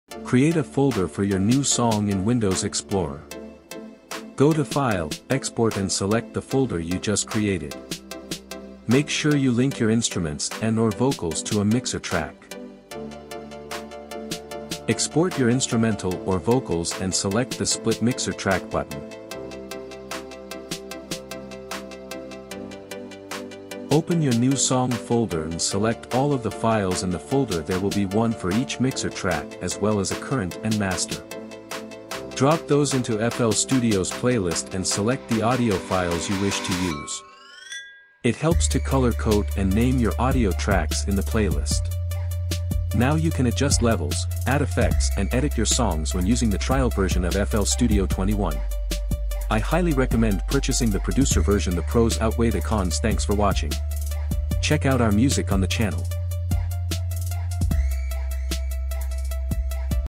FL Studio 21 - How To "Save" with Trial Version
Short Video explaining how to export project split to be able to open it later.
Song playing - Produced by Mammana